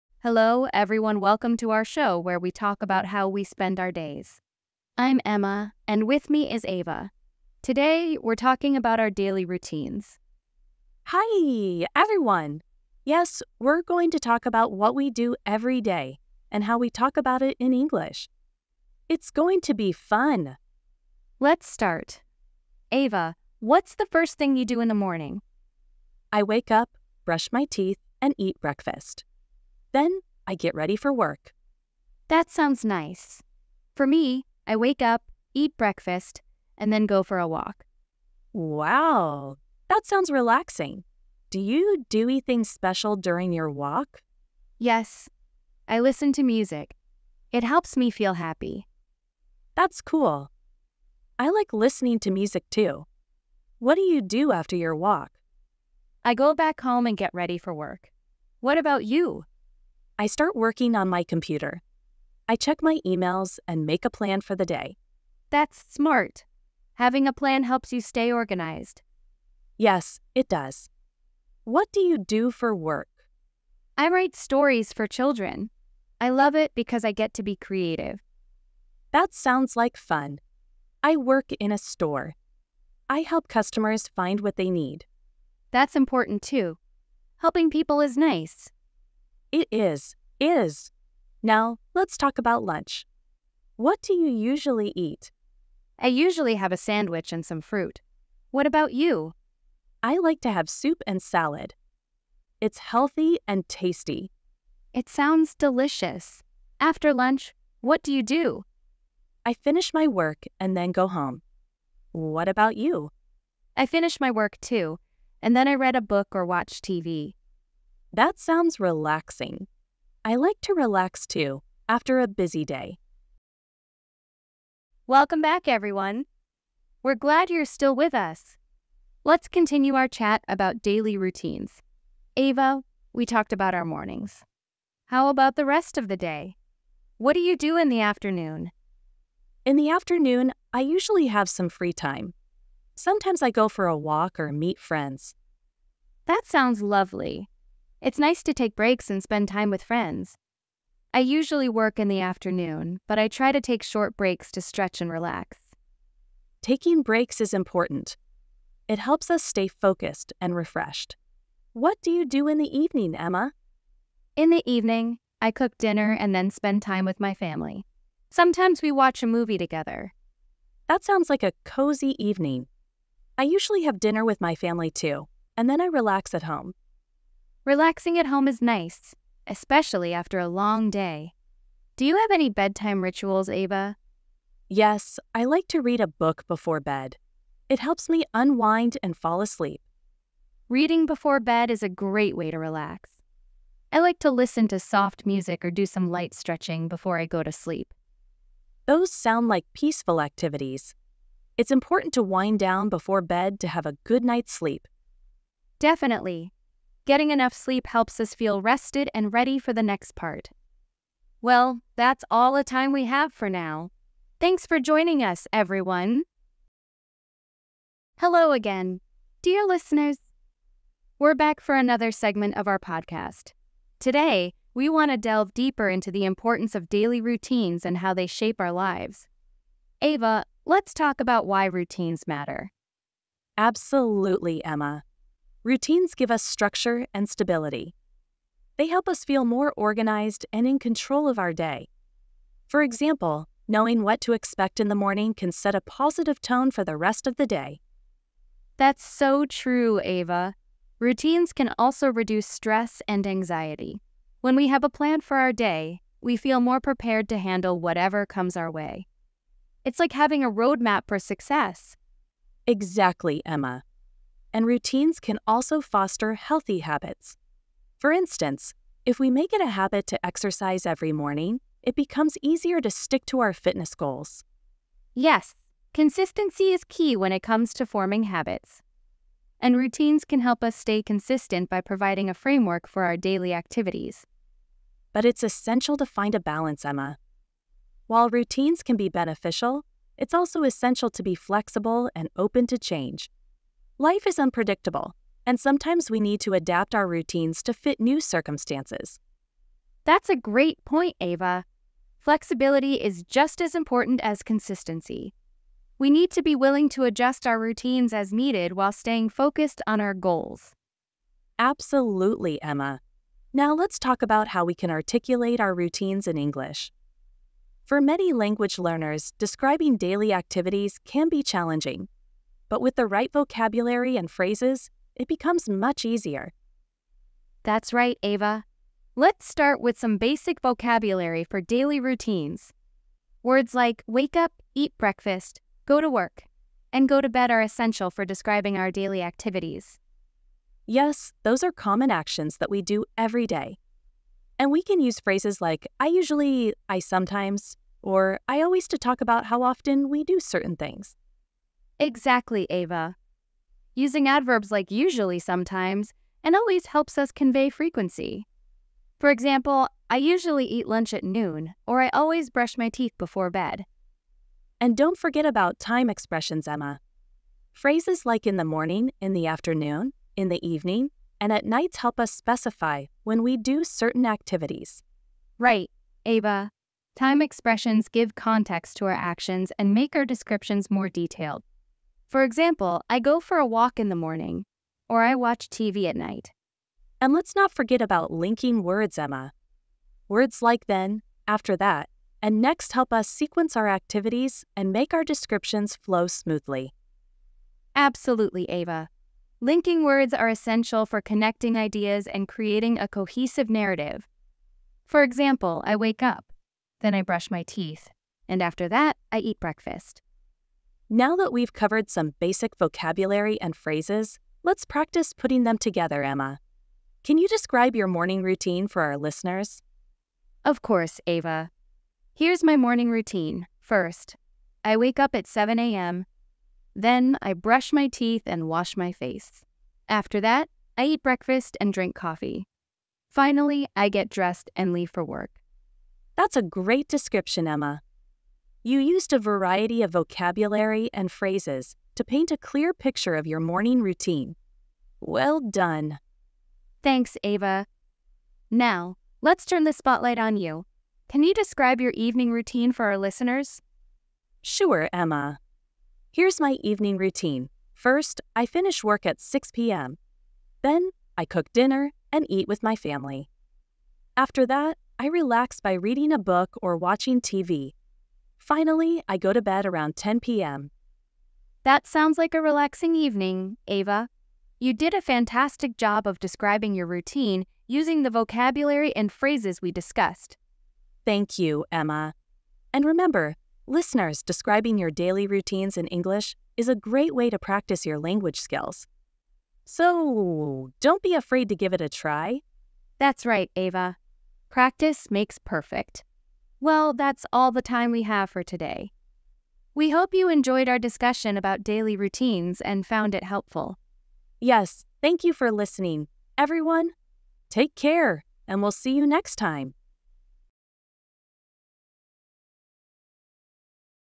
Intermediate English: My Daily Routine | Learn English with Podcast Conversations
In this episode of our podcast, join hosts Emma and Ava as they delve into the fascinating topic of daily routines! 🌞🌜 Whether you're savoring your morning cup of coffee ☕ or winding down with a good book 📚, we'll guide you through the ins and outs of describing your daily activities in English. 🗣️💬

Part - 1: 00:00
Part - 2: 2:04
Part - 3: 3:37

Discover the importance of structure and flexibility in your daily routine, and learn how to articulate your habits with confidence using essential vocabulary and phrases. From waking up bright and early ⏰ to tucking into bed at night 🌙, we'll explore the rhythm of our lives and how to express it effectively in English. 🌟

Join us for engaging conversations, insightful tips, and plenty of language practice in this episode designed for intermediate English learners. 🎧✨ Tune in and take your English skills to the next level! 🚀

Learn English With Podcast Conversation
Podcast for learning English beginners